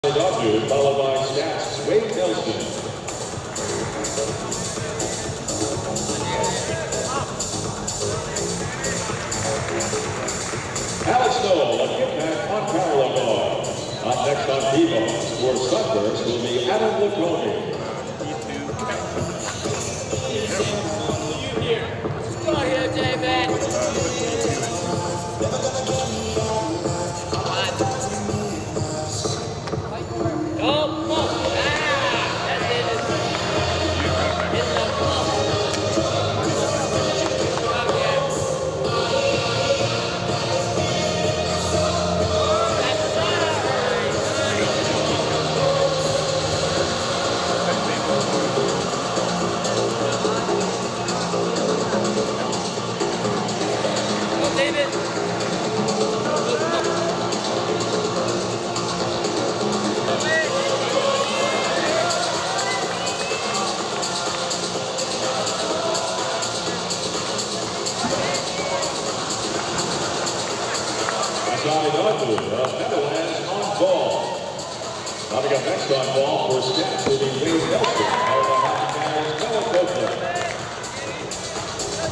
David Ramirez - Still Rings - 2023 Xfinity U.S. Championships - Junior Men Day 1
Aug. 24, 2023 - SAP Center - San Jose, Calif.